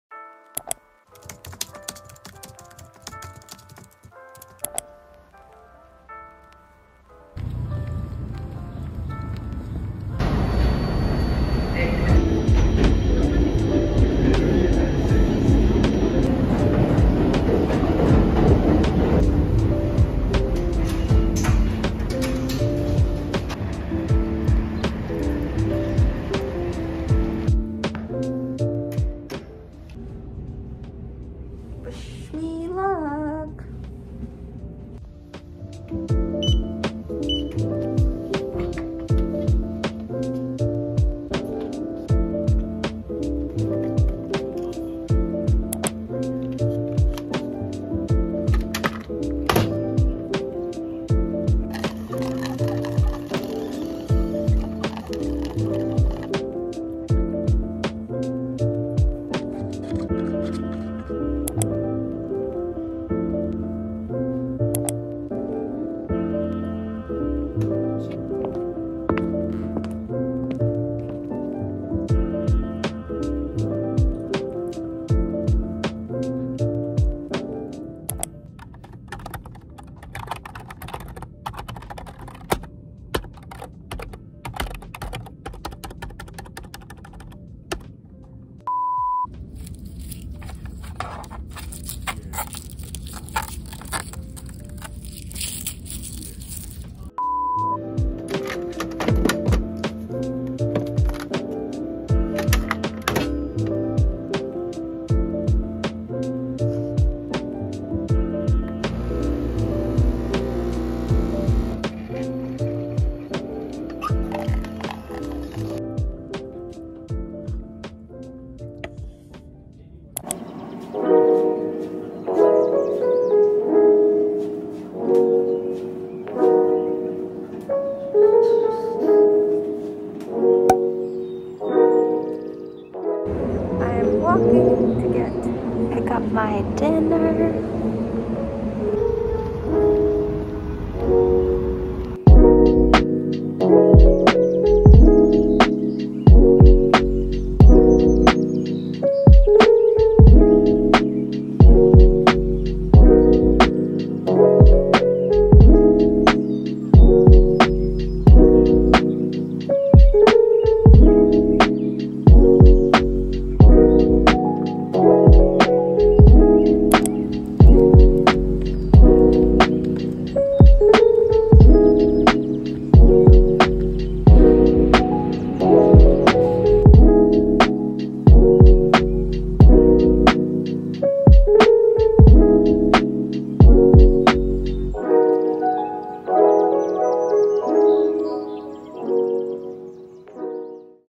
day in the life working in wall street
hi hi! my name is Cleo and i live and work in new york city! in this week's video i show you some quick snippets of my first day in this office since covid started!!